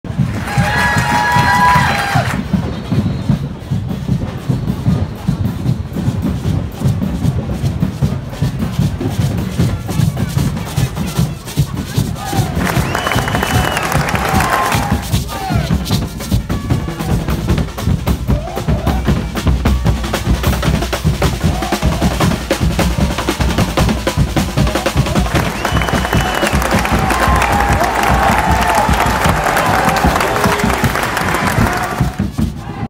外国人欢庆中国年非洲舞 法国巴黎Les étrangers célèbrent le Nouvel An chinois dans le 13ème arrondissement de Paris,
外国人欢庆中国年 非洲舞  地点：法国巴黎13区。 2023年月19日  3Les étrangers célèbrent le Nouvel An chinois Danse africaine Lieu : 13e arrondissement de Paris, France